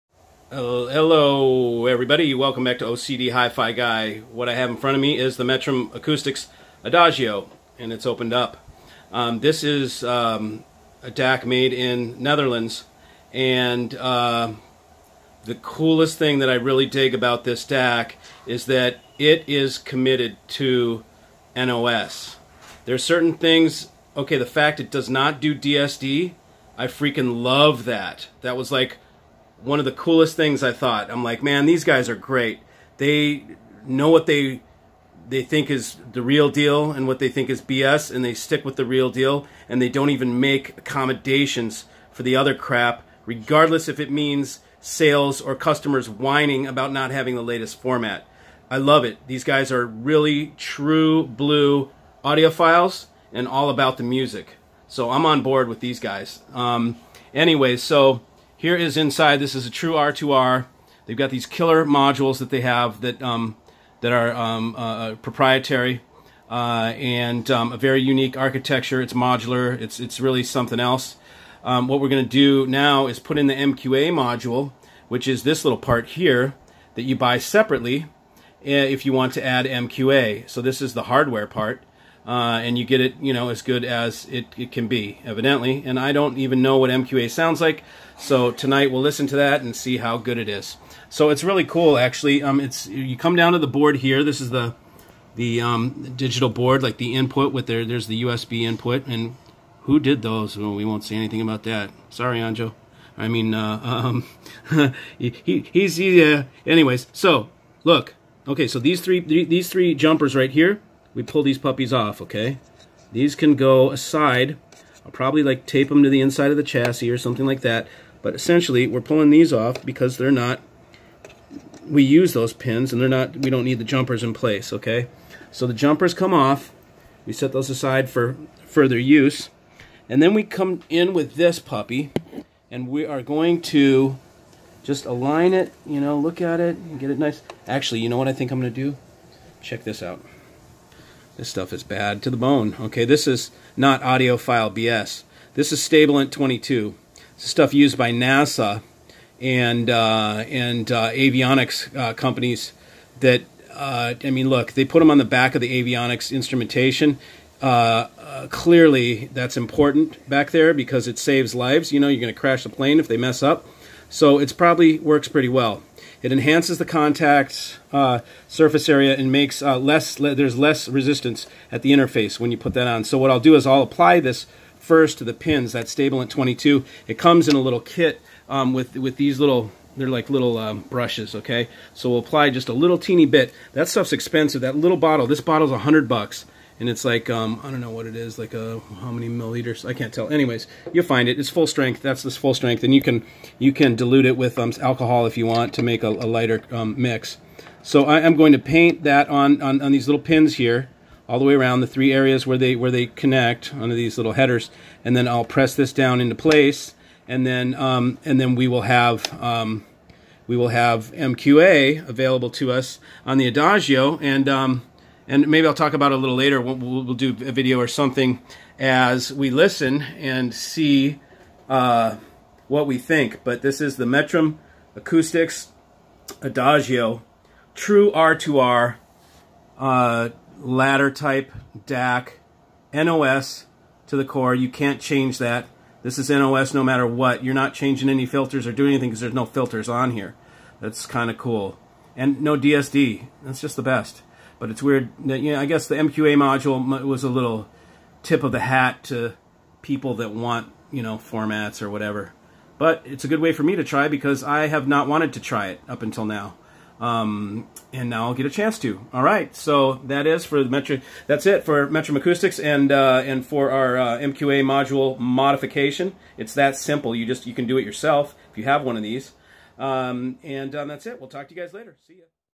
Metrum Acoustics Adagio - MQA upgrade
Here we see how simple it is to add MQA to your Adagio.